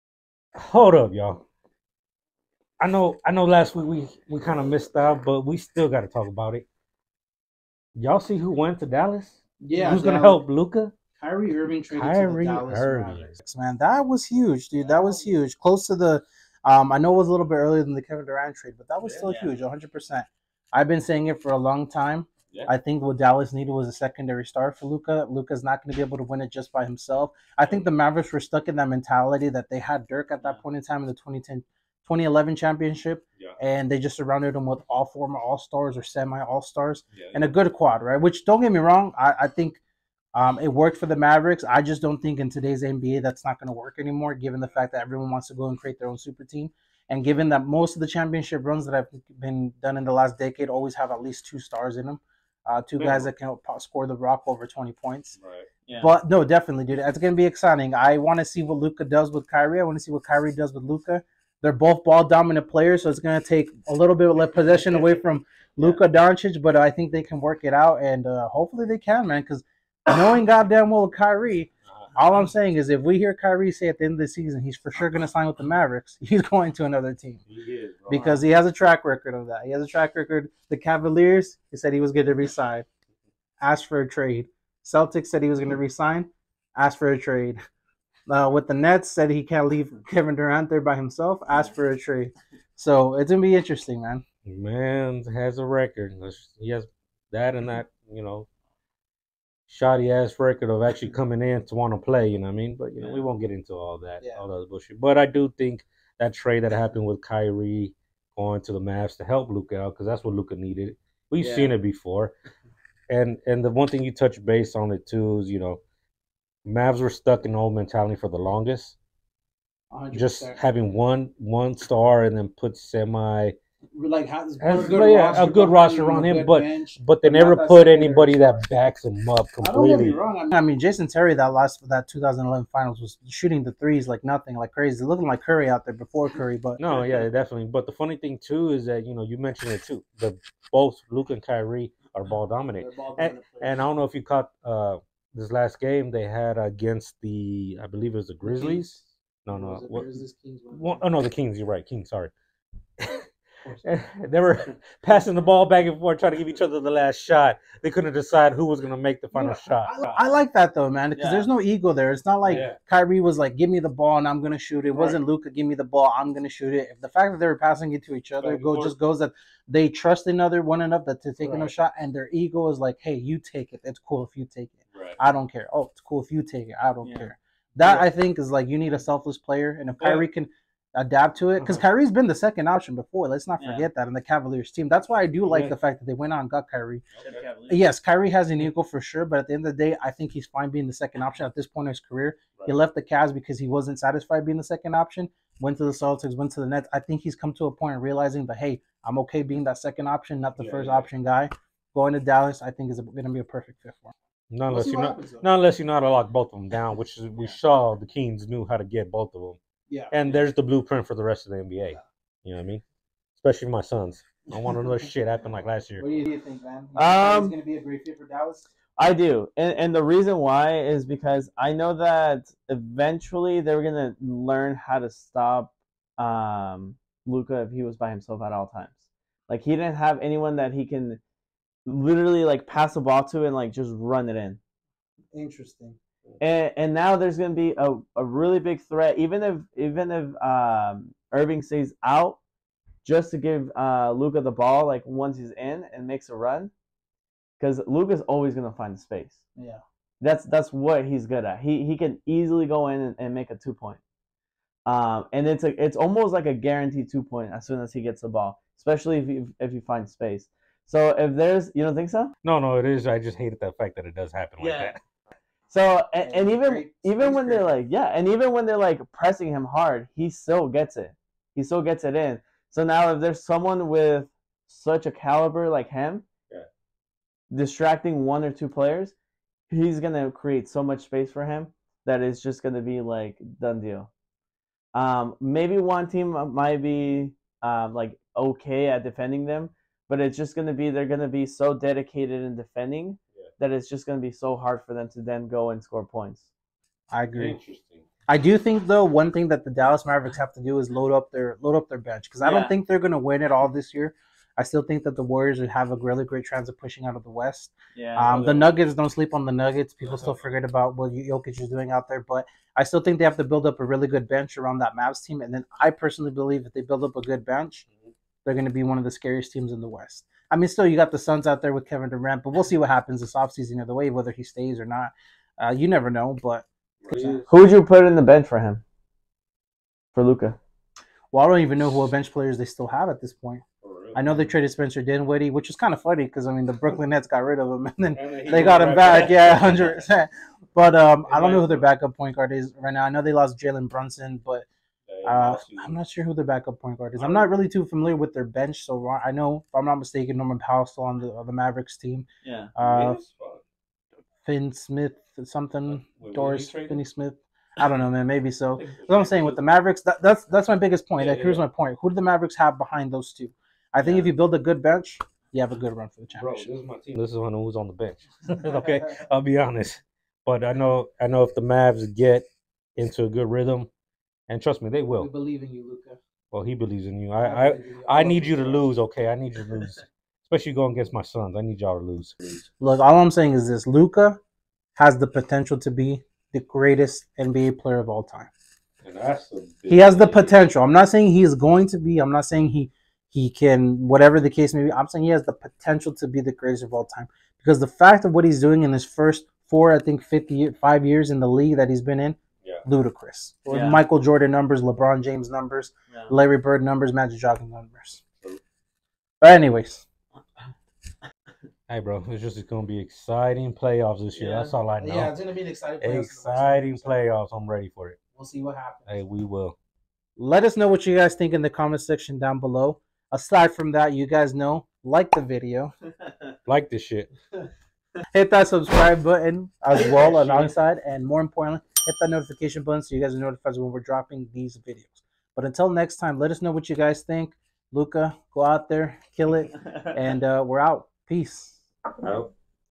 Hol' Up - Kyrie Irving traded to the Mavericks!
Leroy, Short Pipe Miguel, and King Renee discuss Kyrie Irving traded to the Dallas Mavericks.